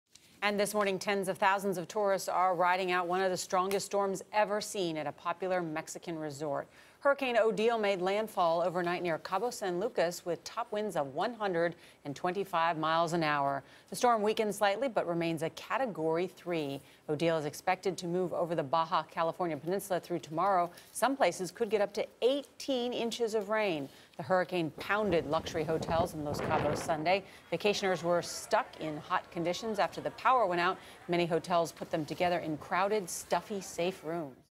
Thousands evacuate in Mexico as Hurricane Odile makes landfall
Thousands of tourists are stuck at a Mexican resort as Hurricane Odile pounds the Cabo San Lucas area. The Category 3 storm is expected to move over the Baja California peninsula through Tuesday. Norah O'Donnell reports.